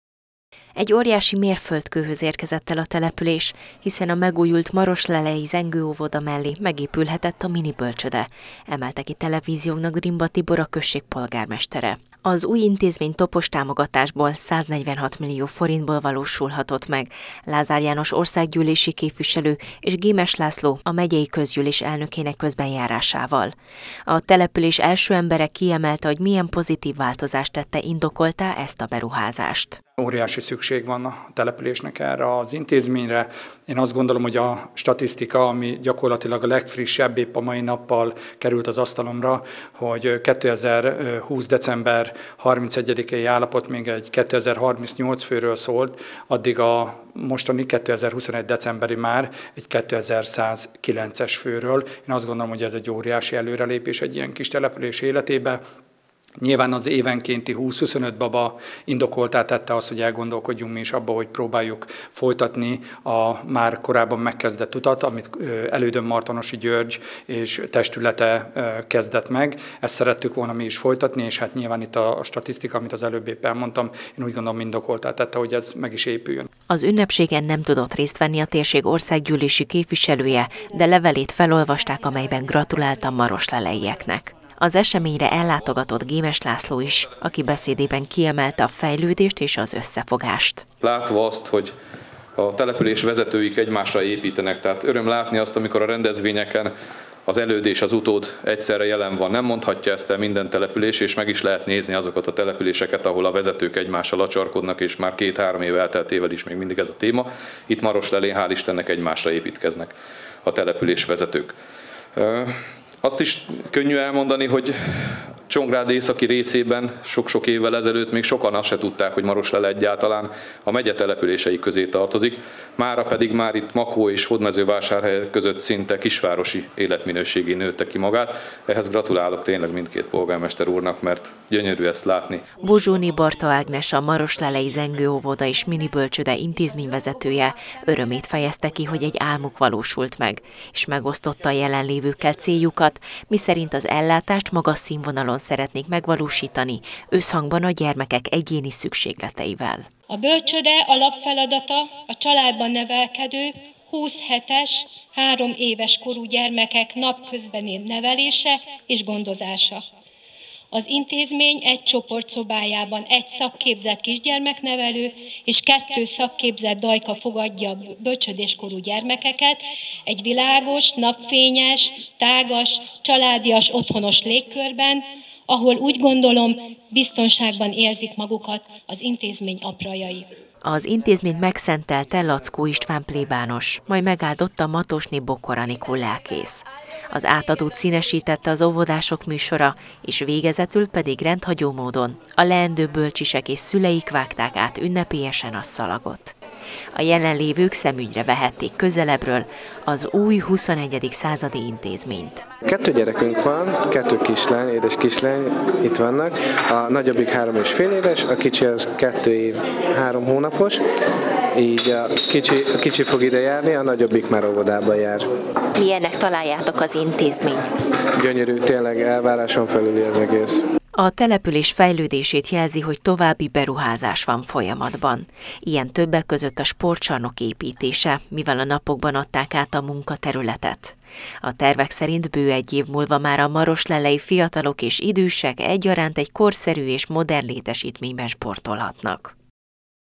Átadták a megújult óvoda mellett épült bölcsődét Maroslelén
Ünnepélyes keretek között adták át tegnap délután Maroslelén az új bölcsődét, amellyel 3 munkahelyet teremtettek és 7 kisgyermek számára biztosítanak nappali ellátást. Az örömteli eseményen köszöntőt mondott Drimba Tibor polgármester, Gémes László a Csongrád-Csanád megyei közgyűlés elnöke, majd a helyi egyházfők megáldották az intézményt.